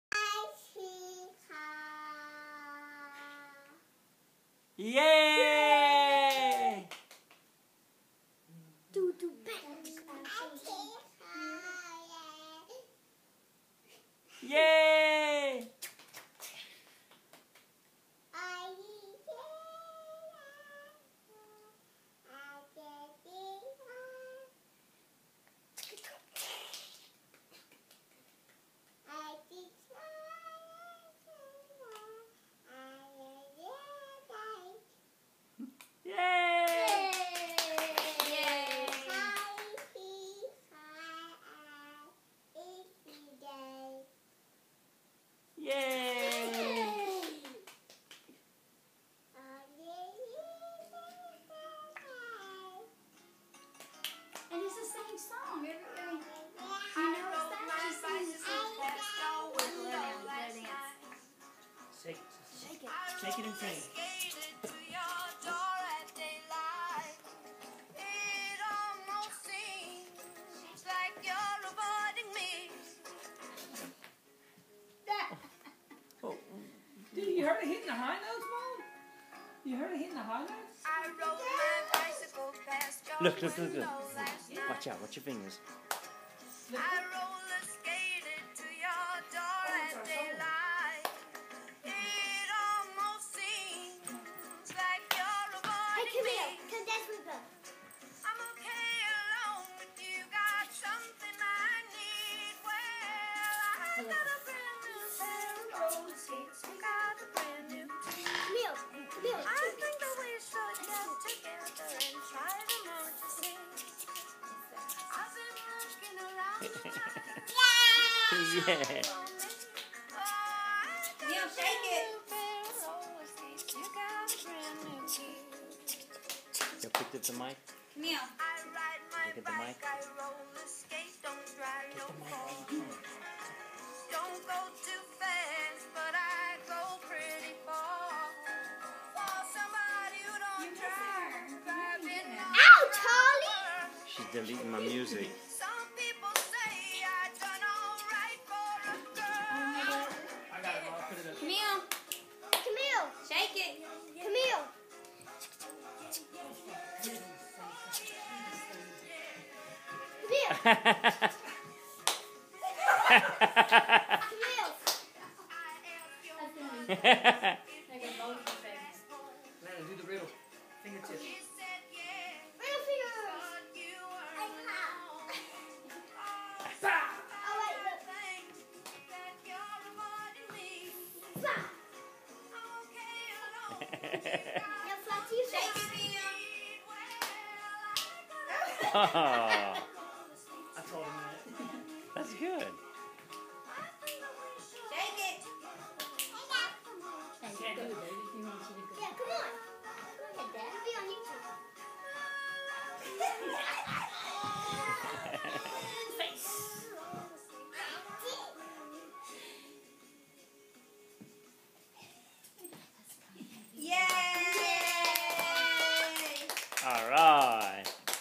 Camille dances and sings
Our granddaughter has some fun.   Her brother and Dad roll on the floor with her.